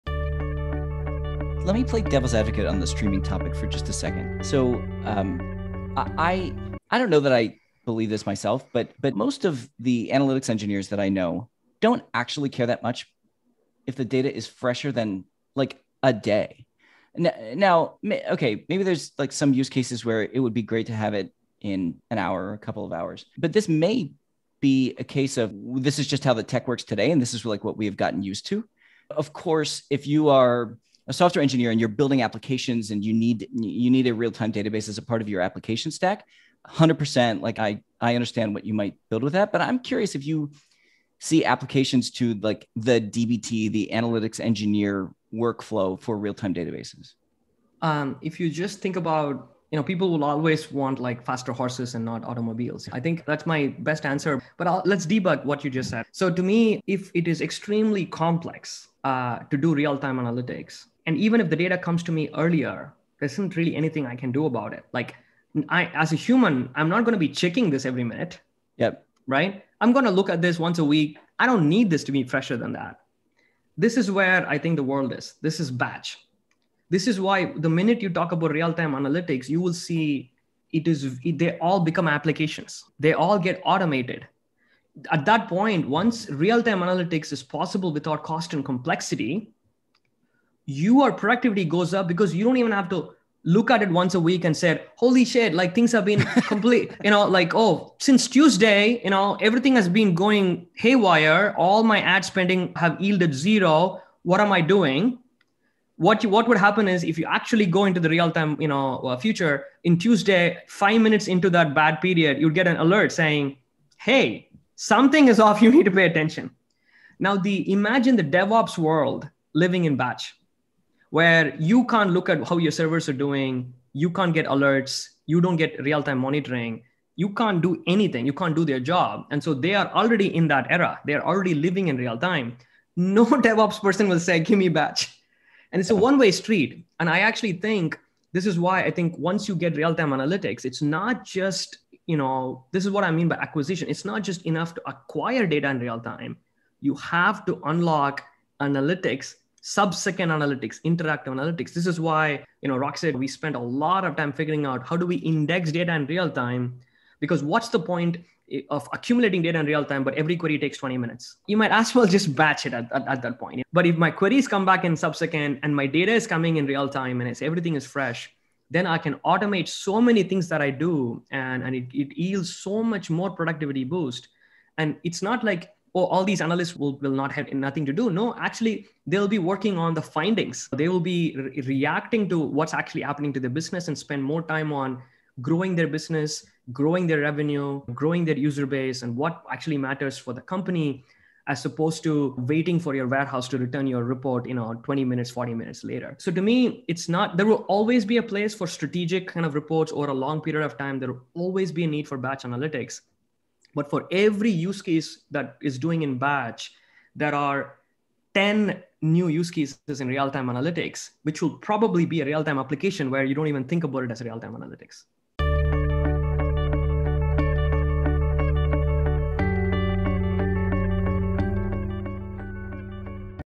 Is Real-time Analytics Really Necessary? Venkat Venkataramani of Rockset Breaks it Down
Venkat knows real-time databases. As Engineering Director at Facebook, Venkat helped build the RocksDB real-time database that powered growth to 5 billion queries per second(!).

Hear him make the case for real-time analytics, and preview use cases that real-time databases unlock to data teams.

Read the show notes & listen to the full conversation with Tristan and Julia